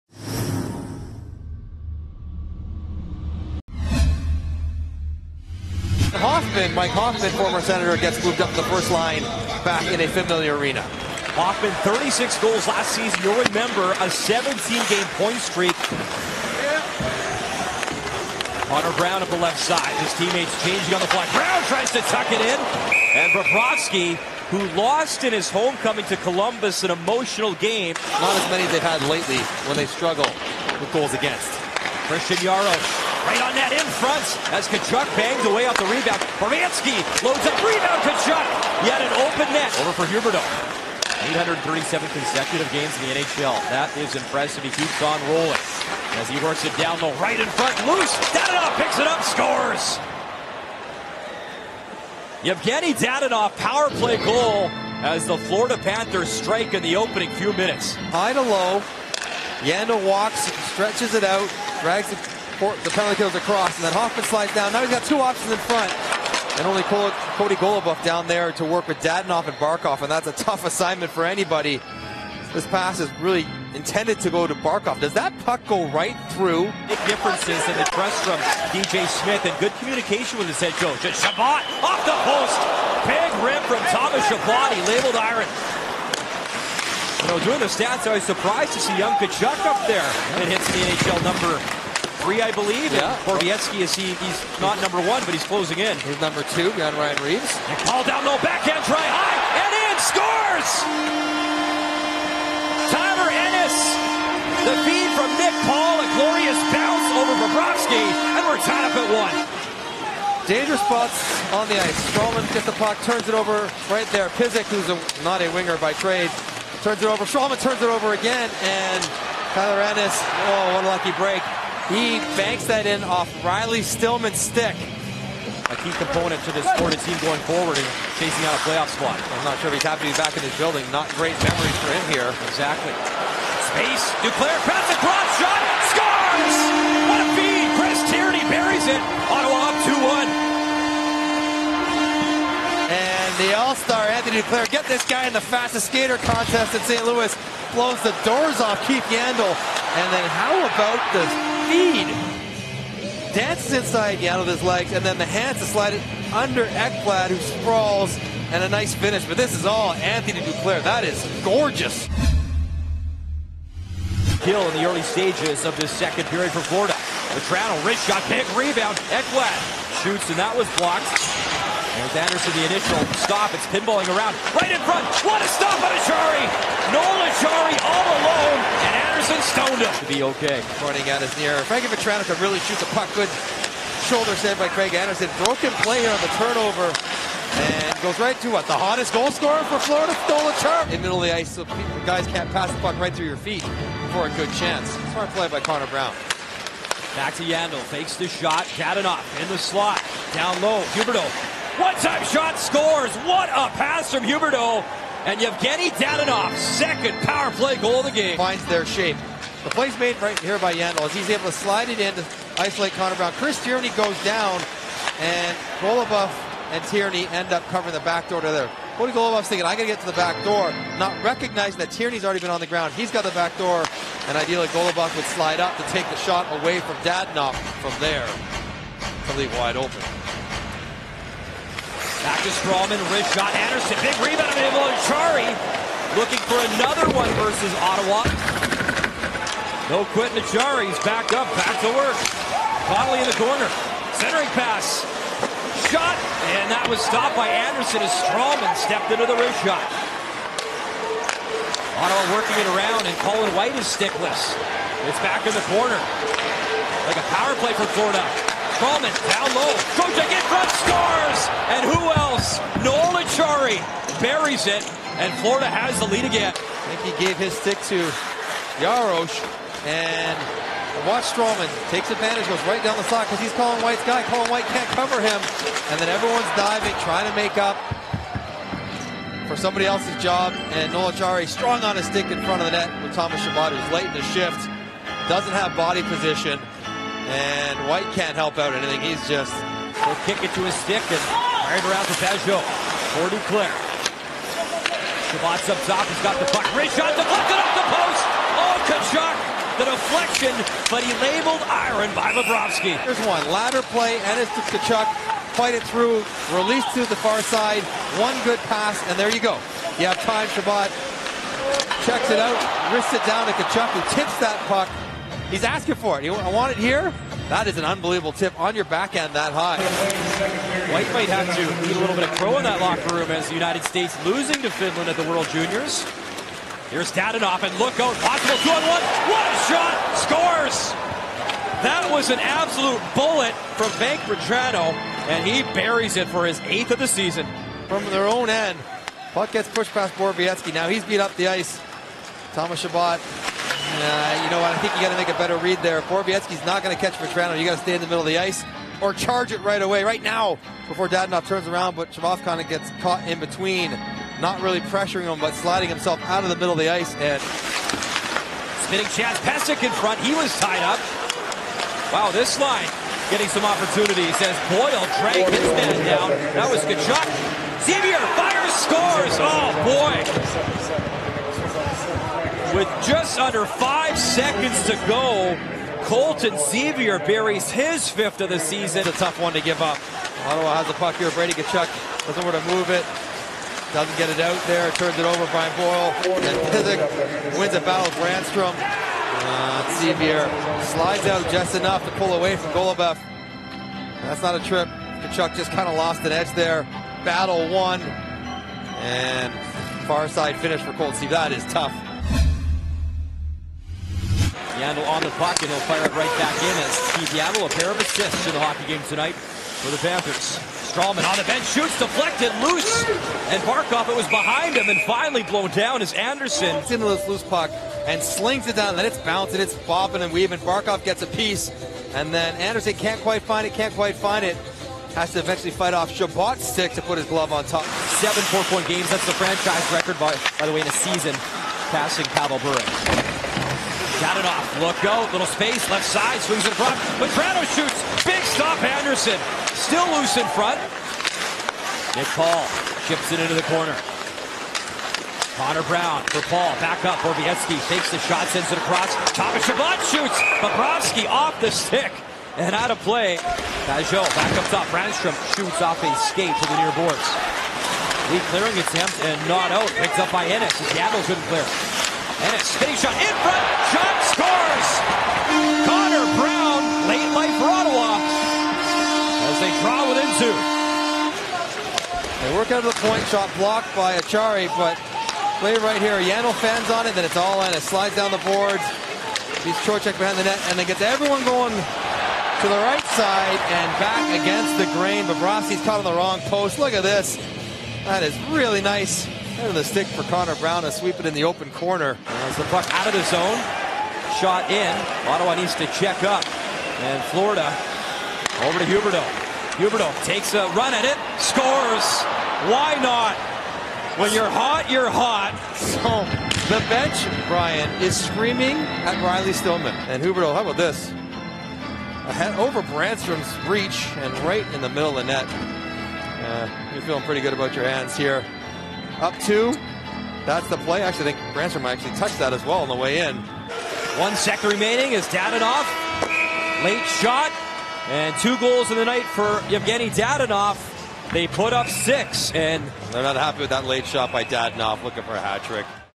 NHL Highlights | Panthers @ Senators 1/2/20
Extended highlights of the Florida Panthers at the Ottawa Senators